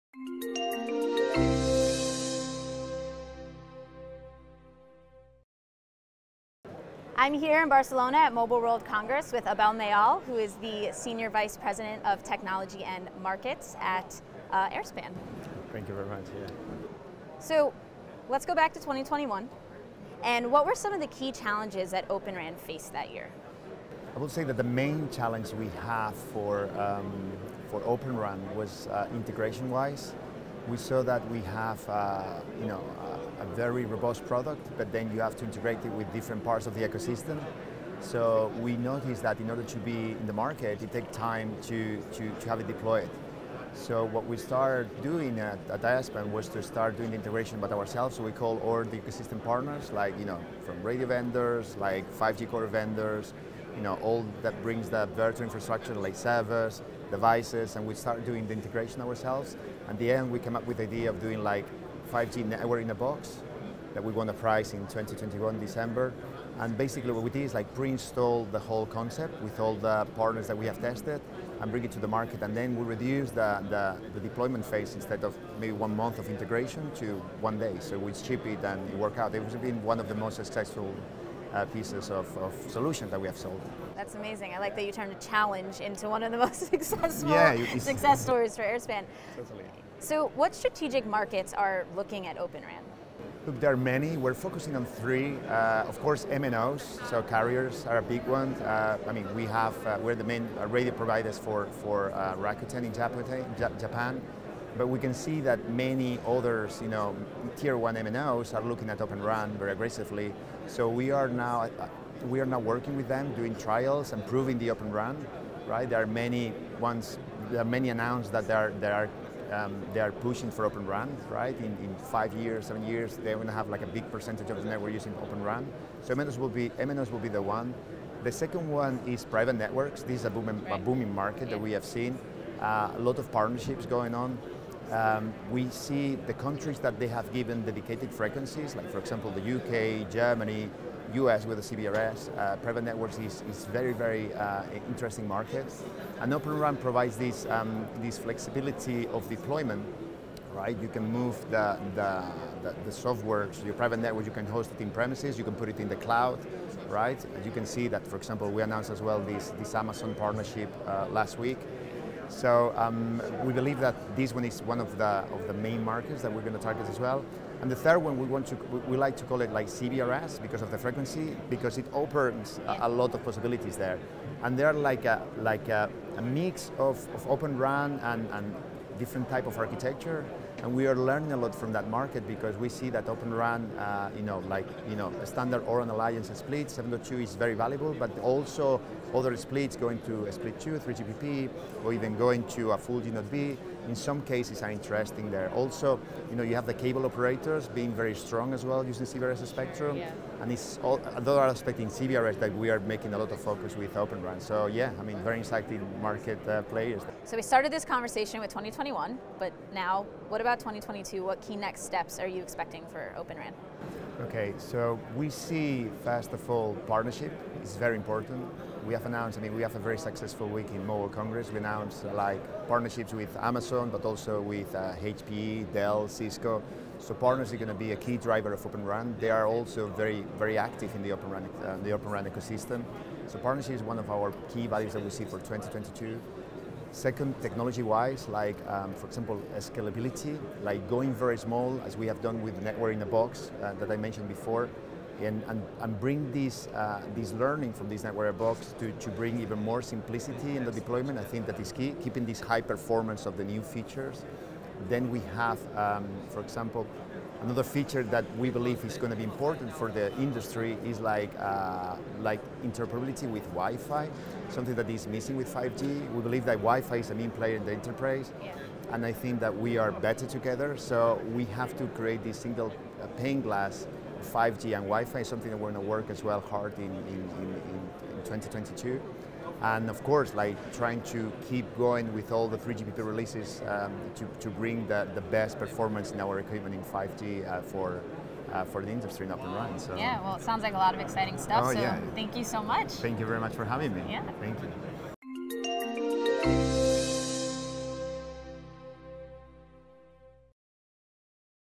How MNOs Are Leveraging Open RAN
Airspan’s SVP of Technology & Marketing Abel Mayal discusses Open RAN and the strategic opportunities for mobile network operators (MNOs) at Mobile World Congress Barcelona 2022